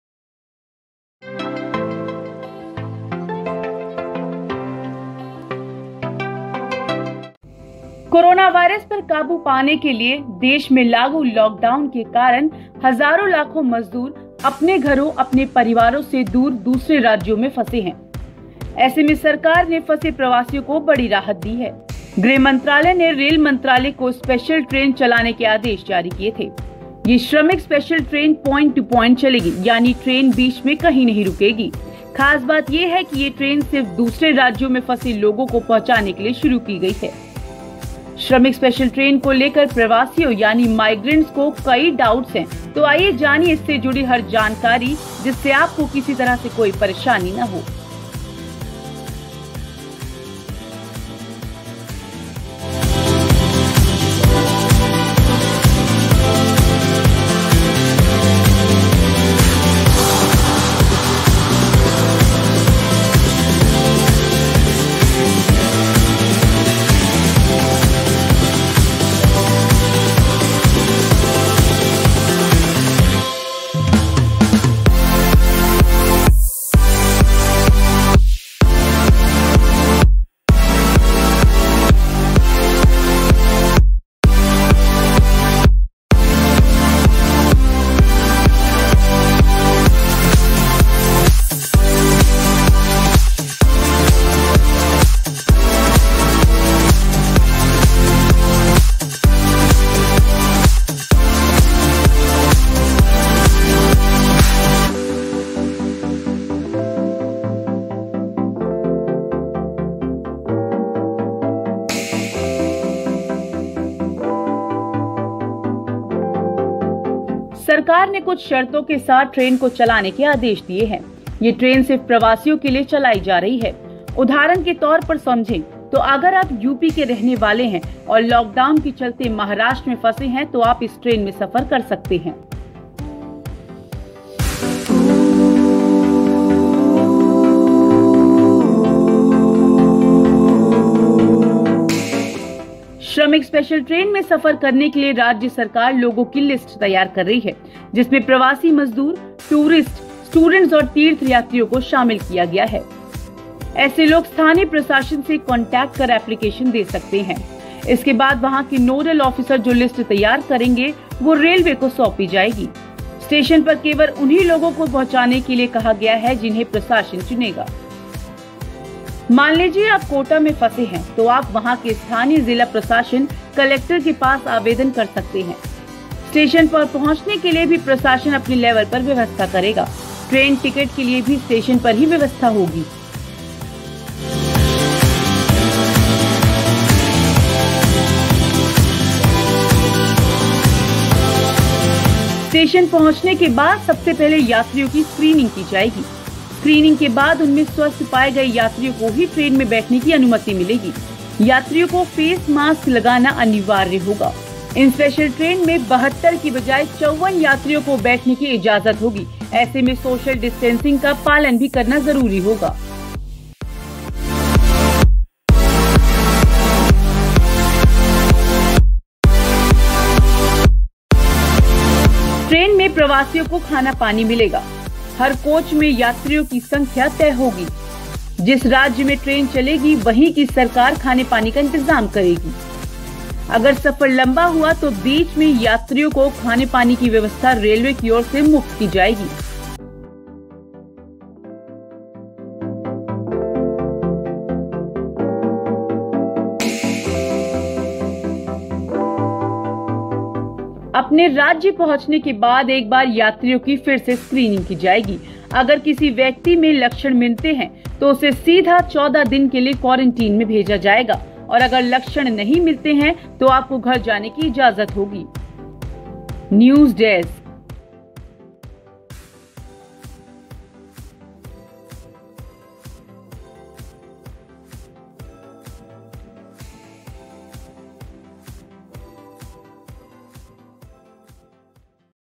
Bihar-Jharkhand जाने वाले प्रवासी दें ध्यान, Shramik Special Train के बारे में जानें सबकुछ
Shramik Special Train को लेकर प्रवासियों यानी माइग्रेंट्स को कई डाउट्स हैं तो आईए जानिए इससे जुड़ी हर जानकारी जिससे आपको किसी तरह की कोई परेशानी ना हो।